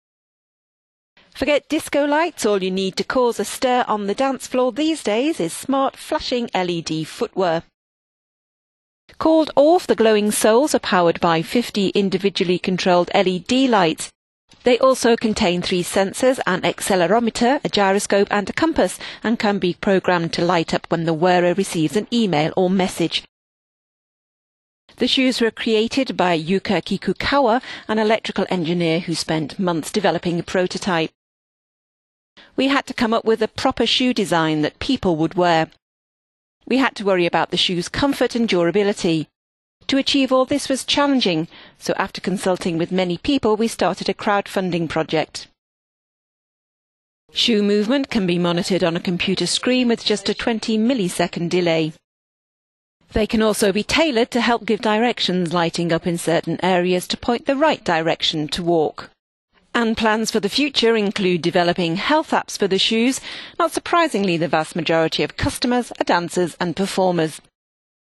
LED shoes take to the dance floor
Forget disco lights. All you need to cause a stir on the dancefloor these days is smart,  flashing LED footwear.

Called Orphe, the glowing soles are powered by 50 individually-controlled LED lights. They contain three sensors; an accelerometer, a gyroscope and a compass and can be programmed to light up when the wearer receives an email or message.

The shoes were created by Yuya Kikukawa, an electrical engineer who spent months developing a prototype.

"We had to come up with a proper shoe d…